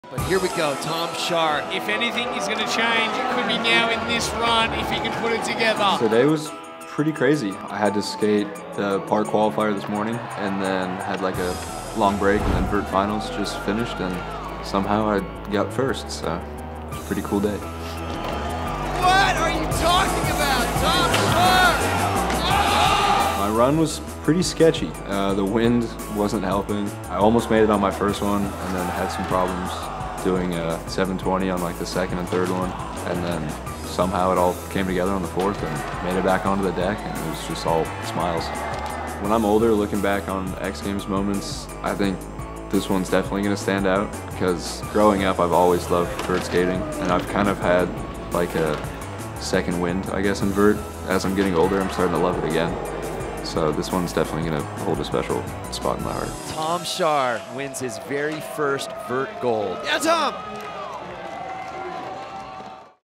Tom Schaar Aims for Another Podium at Games Chiba 2024 | Monster Athlete Profile
Catch Tom Schaar at X Games Chiba in Japan September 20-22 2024!!

An X Games competitor since age 12 at XG Los Angeles 2012, with 12 medals from five different disciplines (Big Air, Big Air Doubles, Vert, Park, MegaPark), Tom Schaar is one of skateboarding's great all-around talents, a bigger-the-better skater known for huge airs and a calm, casual style. His video parts, including the epic 2021 Thrasher Magazine video "Airborne" (filmed partly at the SloanYard MegaPark) have made him equally beloved outside the contest scene. He earned 3 medals in 2023 alone: Skateboard Vert silver at X Games Japan, and bronze in both Park and MegaPark at X Games California. In July 2024, he won Skateboard Vert gold at X Games Ventura, his first win in the discipline; a few weeks later Tony Hawk welcomed him to the Birdhouse Skateboards team with a signature pro deck. In August 2024 he earned a silver medal at the 2024 Paris Olympics, representing USA.

X Games — your destination for all things action sports, music, lifestyle and everything in between.